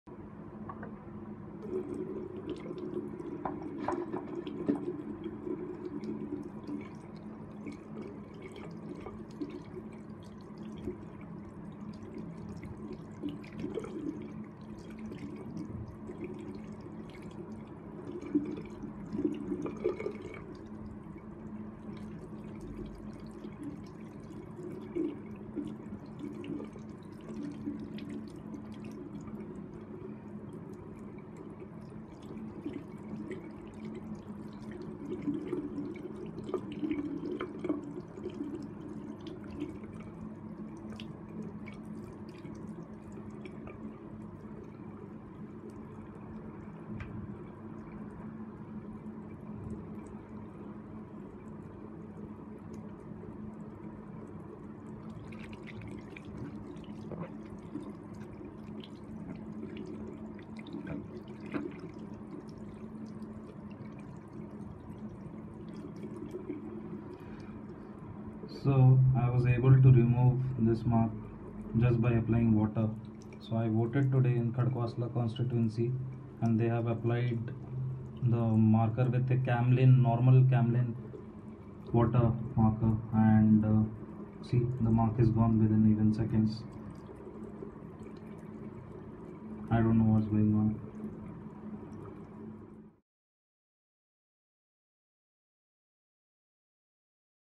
MyReport | My Polling Booth in Pune Used a Marker Instead of Indelible Ink | The Quint
Siji Varghese was excited to vote in the third phase of the 2019 Lok Sabha election from Maharashtra's Baramati, but he was surprised to see the booth official using a marker pen to mark his finger instead of the voting ink.